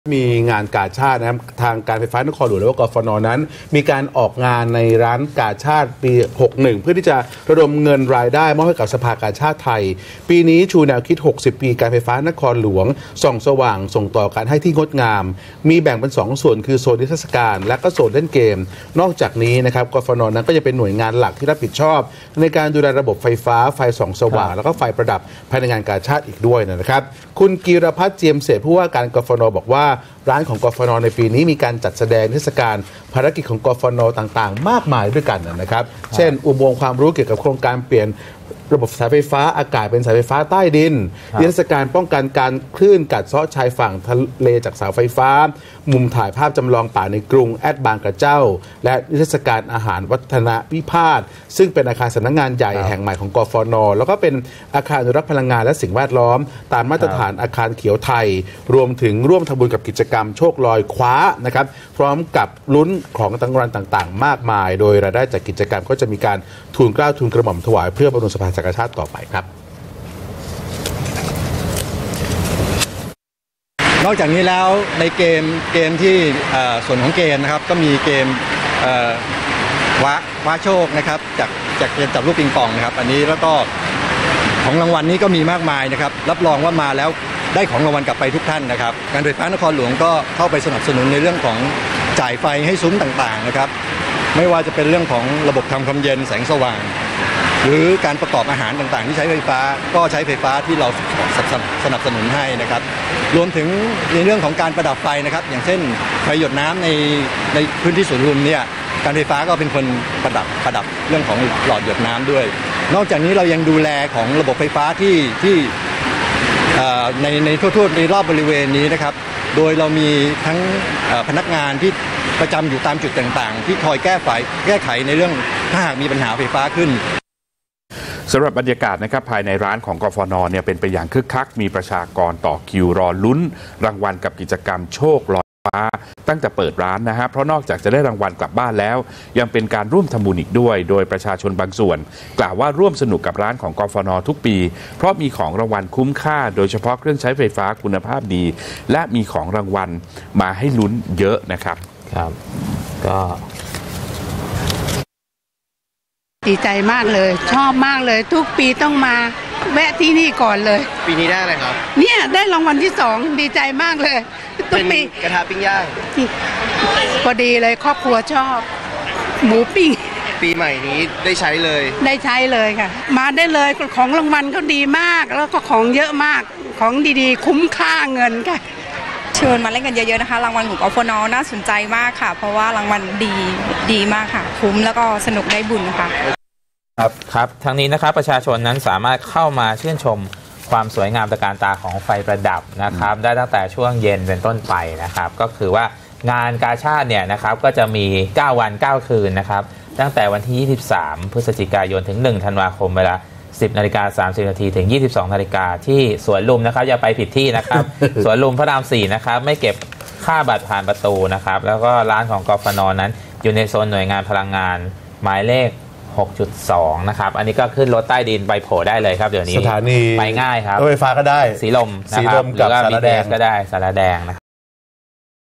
Tonight Thailand - กฟน.ชวนเที่ยวงานกาชาด ร่วมทำบุญลุ้นโชคลอยคว้า
การไฟฟ้านครหลวง หรือ กฟน. ออกร้านงานกาชาด ประจำปี 2561 เพื่อระดมเงินรายได้มอบให้กับสภากาชาดไทย โดยในปีนี้ชูแนวคิด "60 ปี การไฟฟ้านครหลวง ส่องสว่าง ส่งต่อการให้ที่งดงาม" แบ่งเป็น 2 ส่วน คือโซนนิทรรศการ และโซนเล่นเกม นอกจากนี้ กฟน.ยังเป็นหน่วยงานหลักที่รับผิดชอบในการดูแลระบบไฟฟ้า ไฟส่องสว่างและไฟประดับภายในงานกาชาดทั้งหมดอีกด้วย

นายกีรพัฒน์ เจียมเศรษฐ์ ผู้ว่าการ กฟน.กล่าวว่า ร้านของ กฟน. ปีนี้ มีการจัดแสดงนิทรรศการภารกิจของ กฟน. ต่าง ๆ มากมาย เช่น อุโมงค์ความรู้เกี่ยวกับโครงการเปลี่ยนระบบสายไฟฟ้าอากาศเป็นสายไฟฟ้าใต้ดิน นิทรรศการแนวป้องกันคลื่นกัดเซาะชายฝั่งทะเลจากเสาไฟฟ้า มุมถ่ายภาพจำลองป่าในกรุง แอท บางกะเจ้า และนิทรรศการอาคารวัฒนวิภาส ซึ่งเป็นอาคารสำนักงานใหญ่แห่งใหม่ของ กฟน. และเป็นอาคารอนุรักษ์พลังงานและสิ่งแวดล้อม ตามมาตรฐานอาคารเขียวไทย  รวมถึง ร่วมทำบุญกับกิจกรรม "โชคลอยคว้า" พร้อมลุ้นรับของรางวัลต่าง ๆ มากมาย โดยรายได้จากกิจกรรมจะทูลเกล้าทูลกระหม่อมถวายเพื่อบำรุงสภากาชาดไทยต่อไป

สำหรับบรรยากาศ ภายในร้านของ กฟน. เป็นไปอย่างคึกคัก มีประชาชนมารอต่อคิวลุ้นรางวัลกับกิจกรรม "โชคลอยคว้า" ตั้งแต่เปิดร้าน เพราะนอกจากจะได้รางวัลกลับบ้านแล้ว ยังเป็นการร่วมทำบุญอีกด้วย  โดยประชาชนบางส่วน กล่าวว่า ร่วมสนุกกับร้านของ กฟน. ทุกปี เพราะมีของรางวัลคุ้มค่า โดยเฉพาะเครื่องใช้ไฟฟ้าคุณภาพดี และมีของรางวัลมาให้ลุ้นมากมาย

ทั้งนี้ ประชาชนสามารถเข้ามาชื่นชมความสวยงามตระการตาของไฟประดับ ได้ตั้งแต่ช่วงเย็นเป็นต้นไป โดยงานจะมีตลอด 9 วัน 9 คืน ระหว่างวันที่ 23 พฤศจิกายน – 1 ธันวาคม เวลา 10 นาฬิกา 30 นาที - 22 นาฬิกา สวนลุมพินี ถ.พระรามที่ 4 ไม่เก็บค่าบัตรผ่านประตู ซึ่งร้านของ กฟน. อยู่ในโซนหน่วยงานพลังงาน หมายเลข 6.2